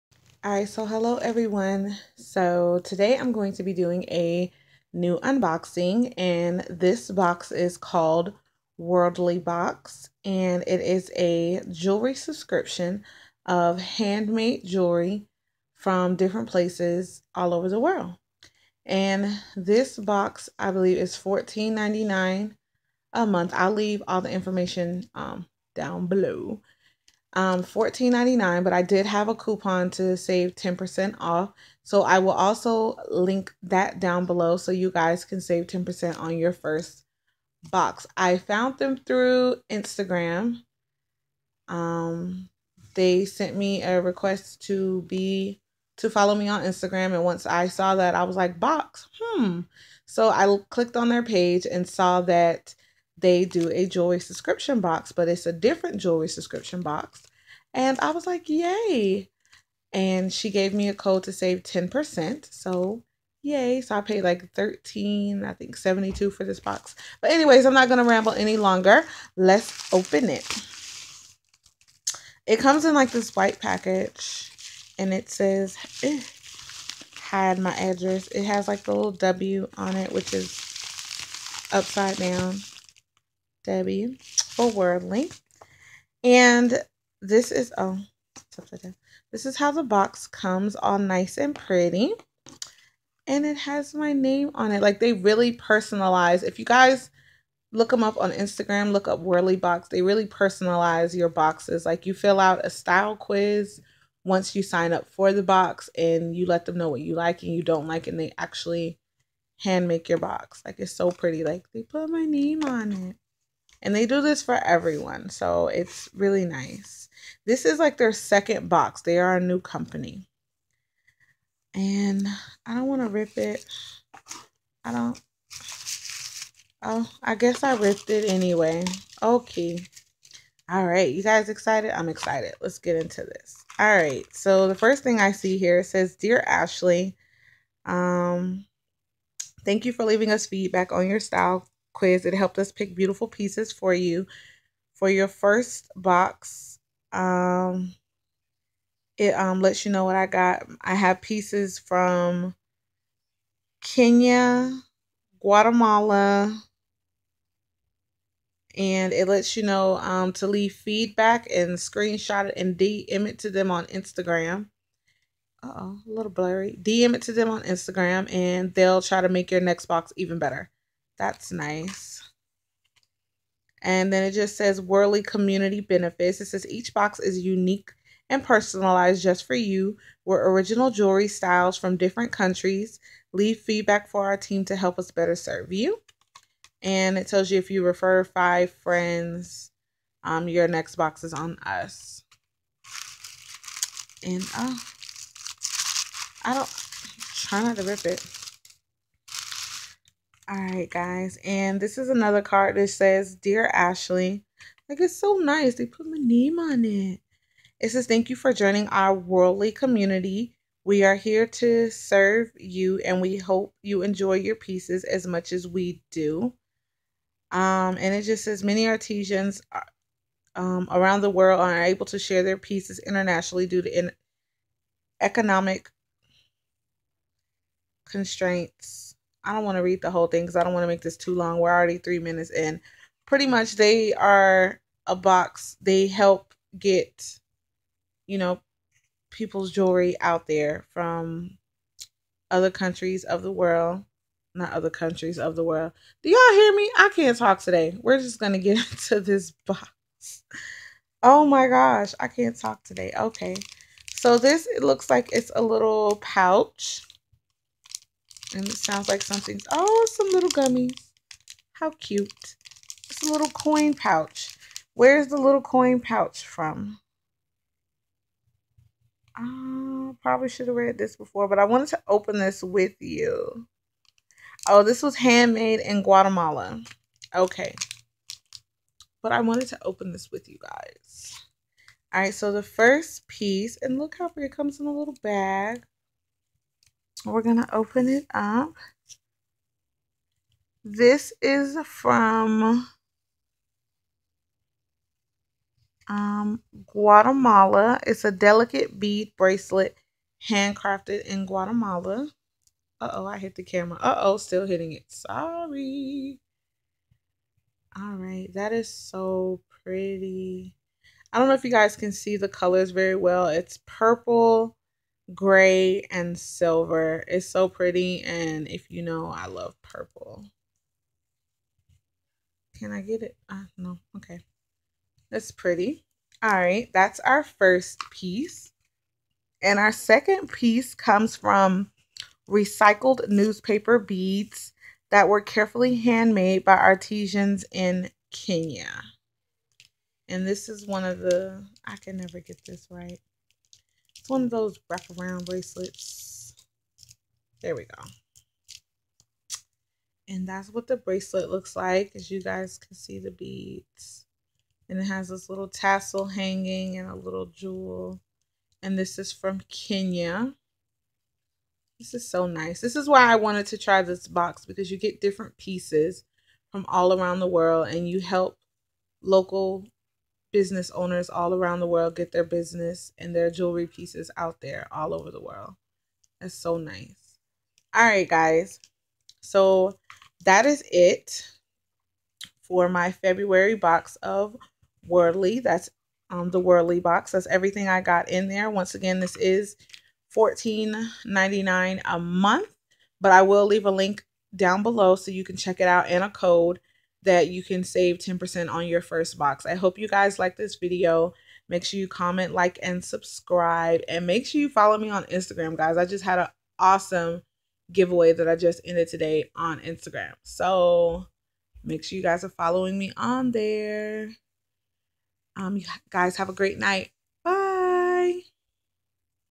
Wordly Box February 2016
This is my very first box and I absolutely love it its $14.99 a month if you want to try here is the link